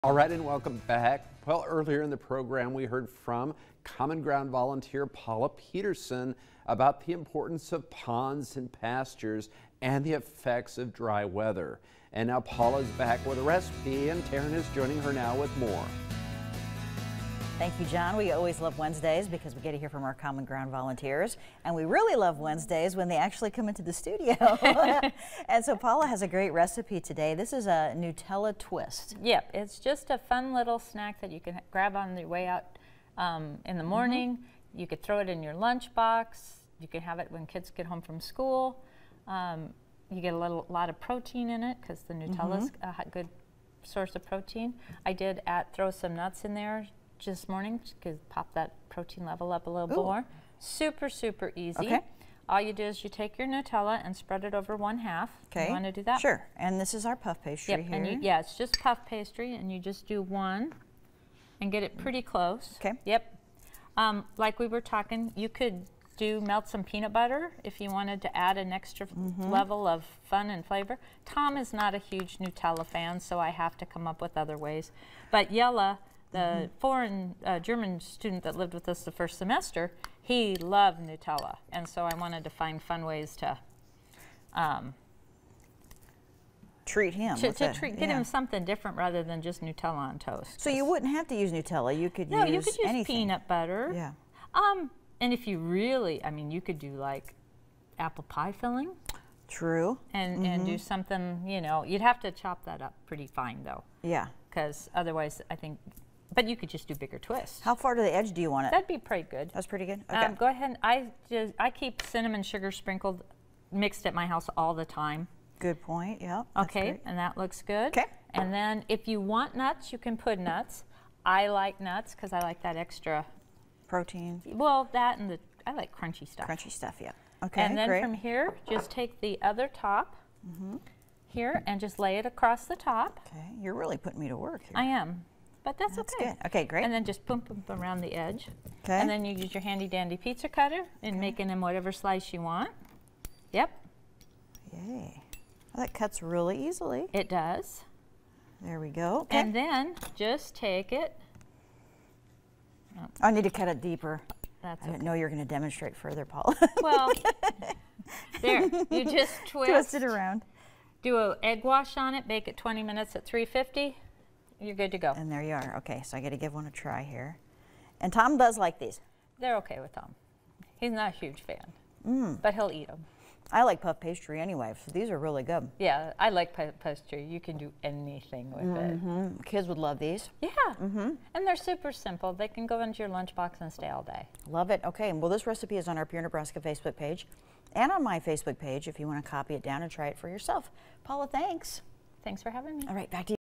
CommonGround Nebraska volunteer Paula Peterson shares her Nutella Twists recipe.
CommonGround Nebraska volunteer Paula Peterson shares her Nutella Twists recipe with Pure Nebraska viewers.